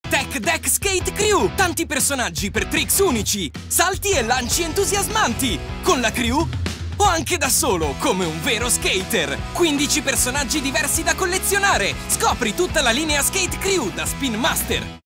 TECH_DECK_SKATE_CREW_15sec
🎉🛹 Ehi piccoli skater! 🛹🎉

La linea Spin Master Tech Deck Crew è finalmente nei negozi Toysland Giocheria! 🙌

 Costruisci il tuo skatepark, fai trick da urlo e diventa un vero campione di skateboarding! 🏆

Le mini tavole da skate Tech Deck sono pronte per farvi divertire ovunque, dalle rampe ai marciapiedi! 😎🔥

Correte da Toysland per mettere alla prova le vostre abilità! 💥